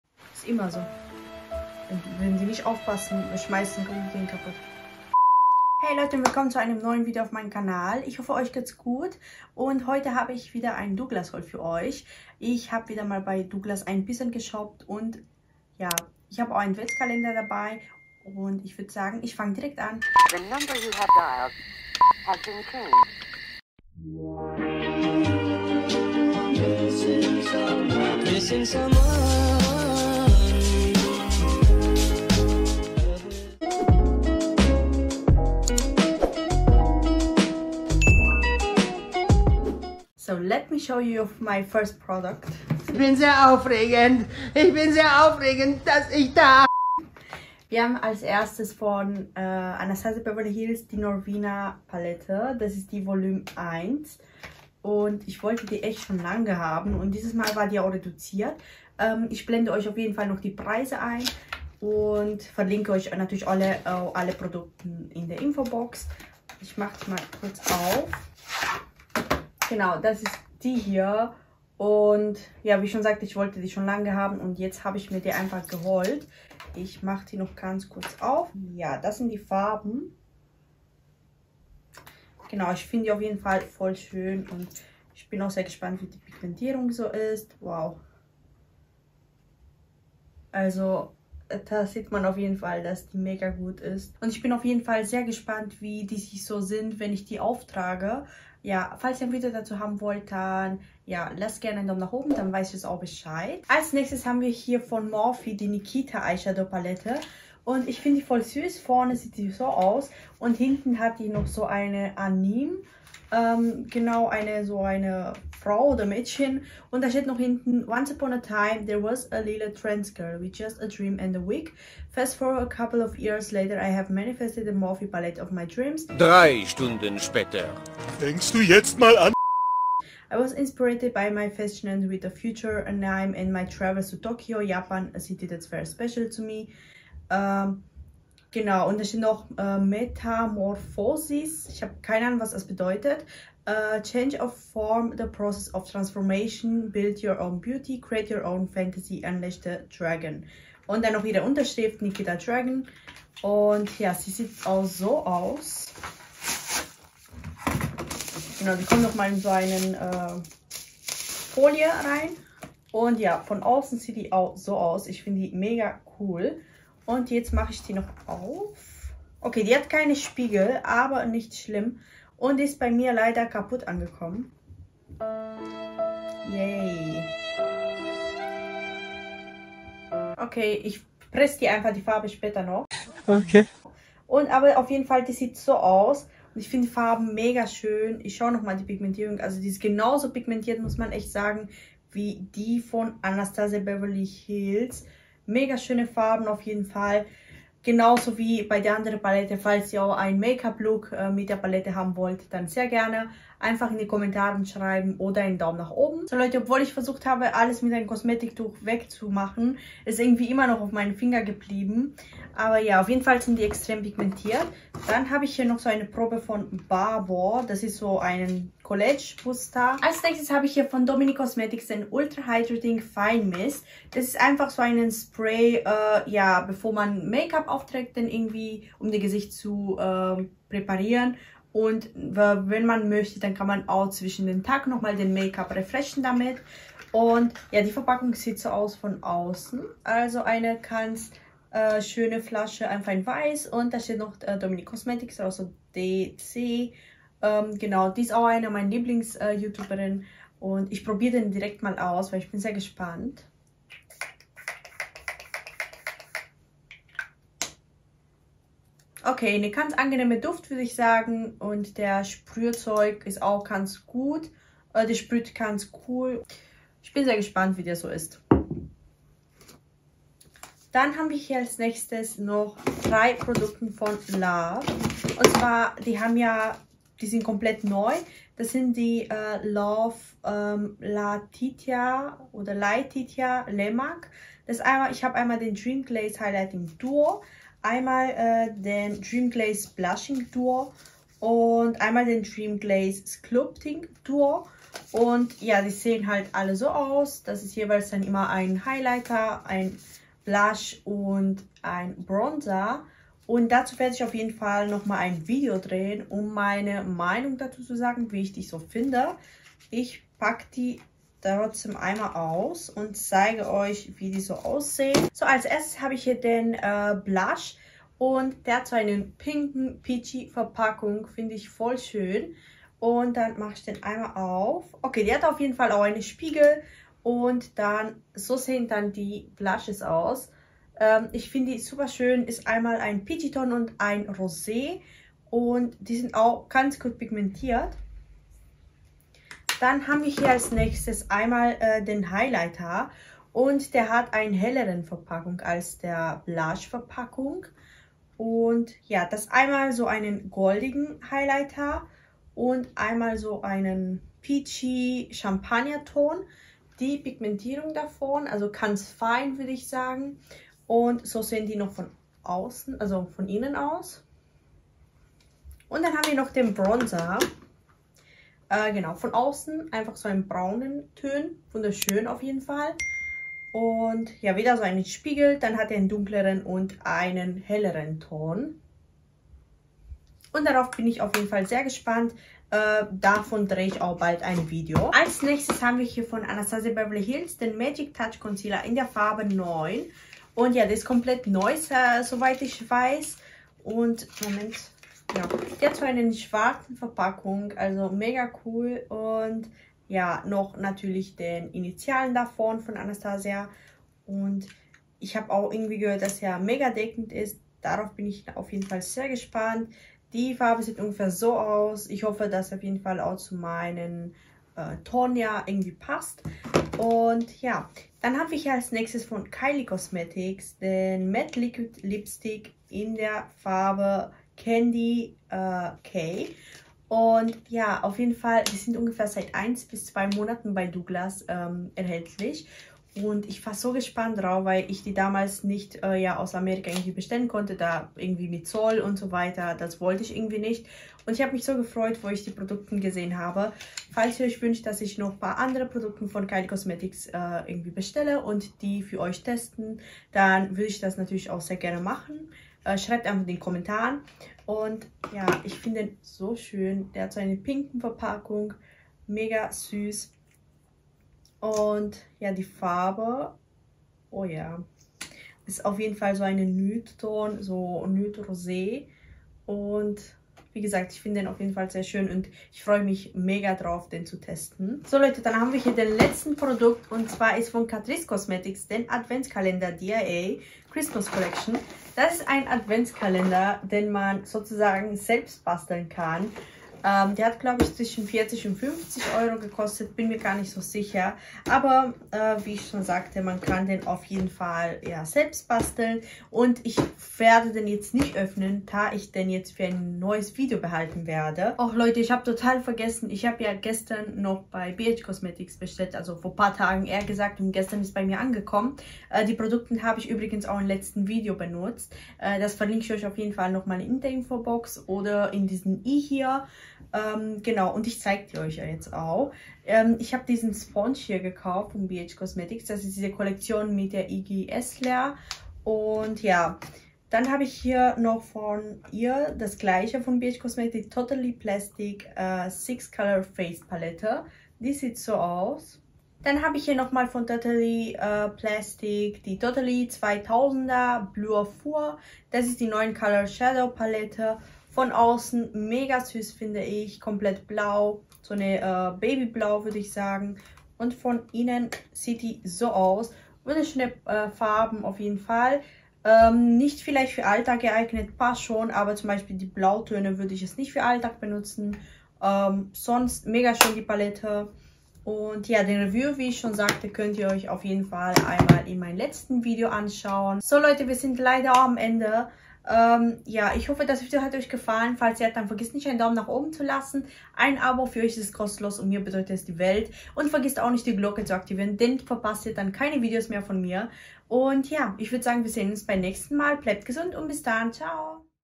XXL Haul- I DID IT AGAIN! (Douglas, BH Cosmetics)
Was geht abbb 🖤

In dem Video könnt ihr  mein XXL Haus sehen , i don´t know why i did it again???   Ich wünsche euch viel spaß dabei 🖤

L.O.V Puder L.O.V x LAETITIA LEMAK Dream Glaze Blushing Duo

L.O.V Puder L.O.V x LAETITIA LEMAK Dream Glaze Highlighting Duo

L.O.V Puder L.O.V x LAETITIA LEMAK Dream Glaze Sculpting Duo